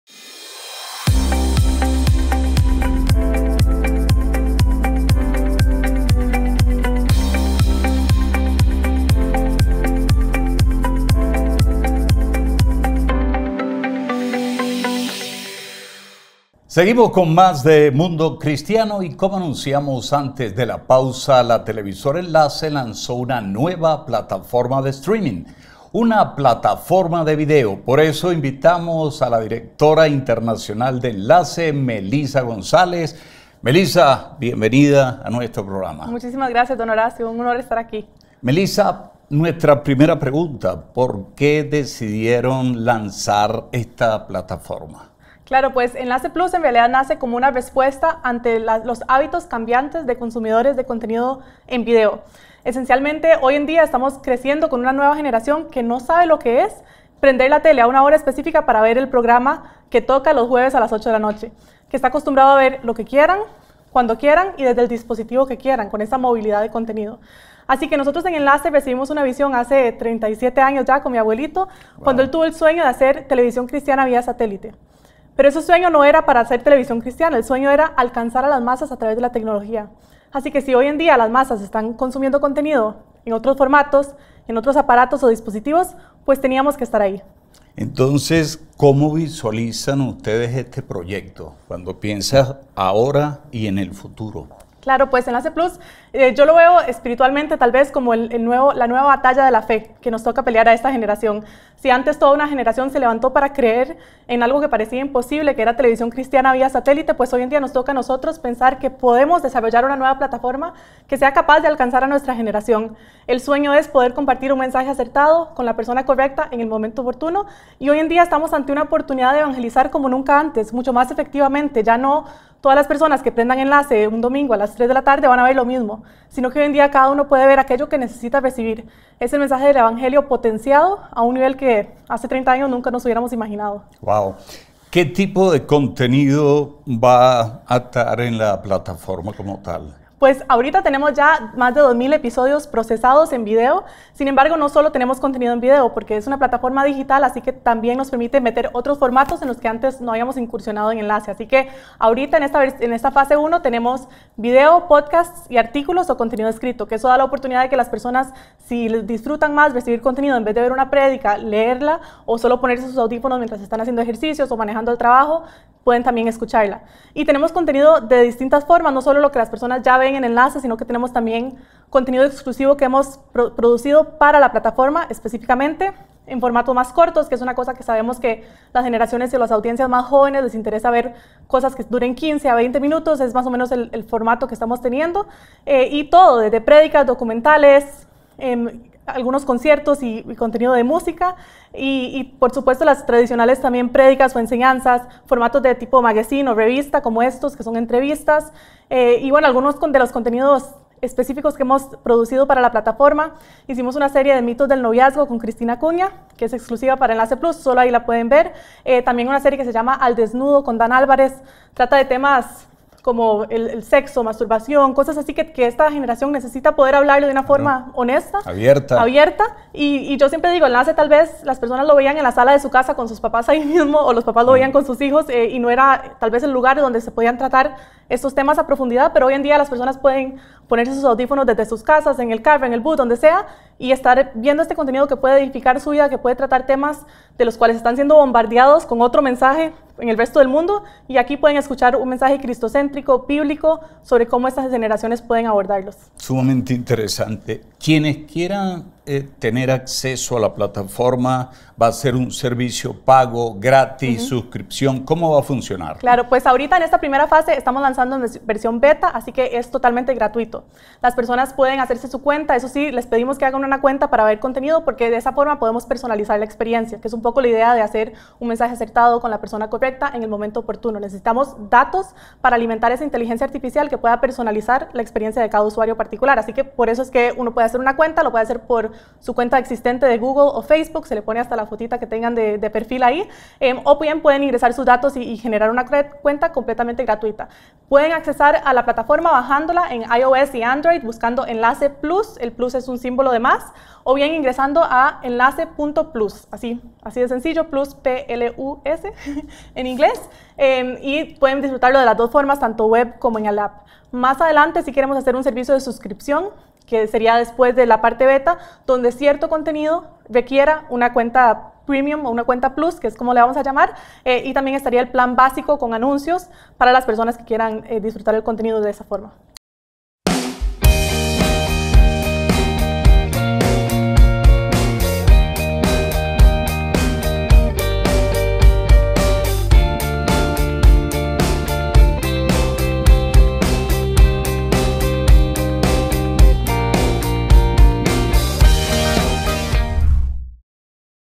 Nueva Plataforma de streaming cristiana
La televisora cristiana Enlace, ha lanzado oficialmente una plataforma de video que permitirá a los usuarios acceder a todo tipo de contenido para la iglesia. se trata de Enlace Plus una plataforma de streaming que contiene prédicas, documentales, contenido musical, programación para niños, noticias entre otros tipos de material audiovisual con contenido cristiano.